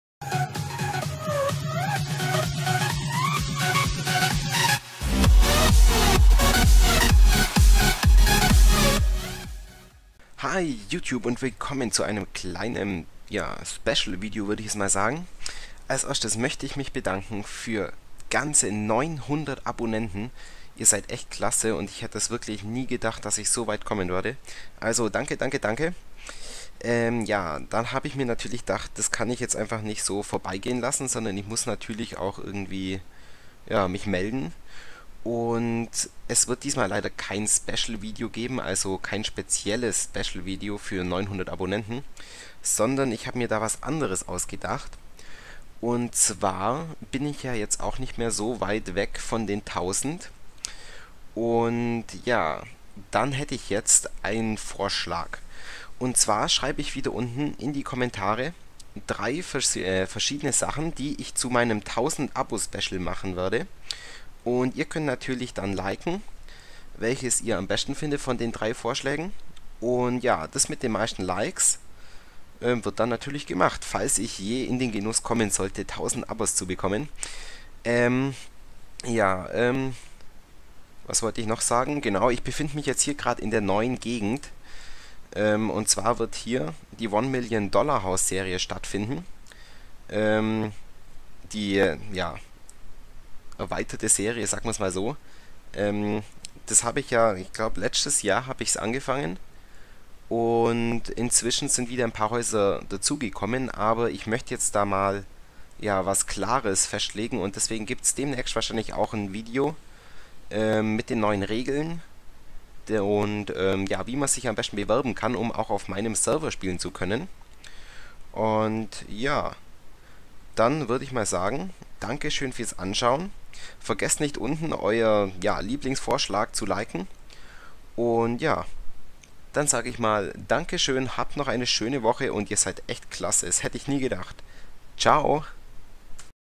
900 Abo-Special (ich sage Danke) :D
900 Abos, ich kanns immer noch nicht glauben und nochmals vielen Dank, Ihr seid echt Klasse :DD

Texture Packt ist das FlowsHD für die 1.7.4